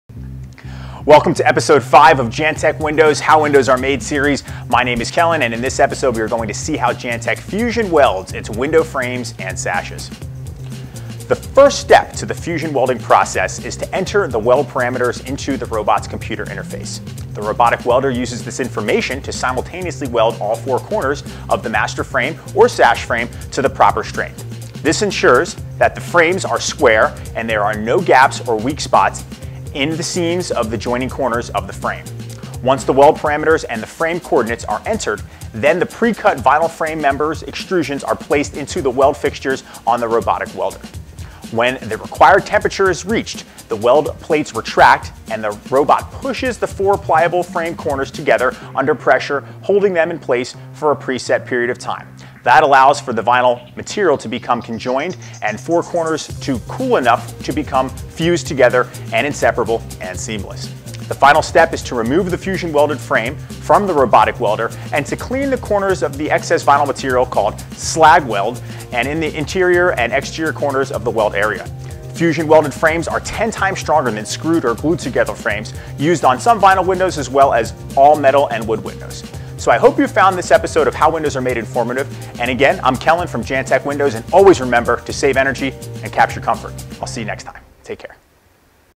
How Vinyl Windows Are Welded
IN THIS EPISODE OF HOW VINYL WINDOWS ARE MADE,  WE DEMONSTRATE HOW VINYL WINDOWS ARE WELDED.

THE FIRST STEP TO THE FUSION WELDING PROCESS IS TO ENTER THE WELD PARAMETERS INTO THE ROBOT’S COMPUTER INTERFACE. THE ROBOTIC WELDER USE THIS INFORMATION TO SIMULTANEOUSLY WELD ALL FOUR CORNERS OF THE WINDOW MASTER FRAME OR THE SASH FRAME TO THE PROPER STRENGH. THIS ENSURES THAT THE FRAMES ARE SQUARE AND THERE ARE NO GAPS OR WEAK SPOTS IN THE SEAMS OF THE JOINING CORNERS OF THE FRAME.

 ONCE THE WELD PARAMETERS AND THE FRAME COORDINATES ARE ENTERED, THEN THE PRE-CUT VINYL FRAME MEMBER EXTRUSIONS ARE PLACED INTO THE WELD FIXTURES ON THE ROBOTIC WELDER.

WHEN THE REQUIRED TEMPERATURE IS REACHED THE WELD PLATES RETRACT AND THE ROBOT PUSHES THE FOUR PLYABLE FRAME CORNERS TOGETHER UNDER PRESSURE HOLDING THEM IN PLACE FOR A PRESET PERIOD OF TIME THAT ALLOWS FOR THE VINYL MATERIAL TO BECOME COJOINED AND FOR CORNERS TO COOL ENOUGH TO BECOME FUSED TOGETHER AND INSEPARABLE AND SEAMLESS.

THE FINAL STEP IS TO REMOVE THE FUSION WELDED FRAME FROM THE ROBOTIC WELDER AND TO CLEAN THE CORNERS OF THE EXCESS WELDED VINYL WINDOW MATERIAL, CALLED WELD SLAG, AT THE INTERIOR AND EXTERIOR CORNERS OF THE WELD AREA.

FUSION WELDED WINDOW FRAMES ARE 10 TIMES STRONGER THAN MECHANICALLY FASTENED WINDOW FRAMES THAT ARE SCREWED OR GLUED TOGETHER, USED ON SOME VINYL,  AS WELL AS ALL METAL AND WOOD WINDOWS.

SO I HOPE YOU HAVE FOUND THIS EPISODE OF HOW WINDOWS ARE MADE INFORMATIVE.

AGAIN, I’M KELAN FROM JANTEK WINDOWS… AND REMEMBER… SAVE ENERGY… CAPTURE COMFORT!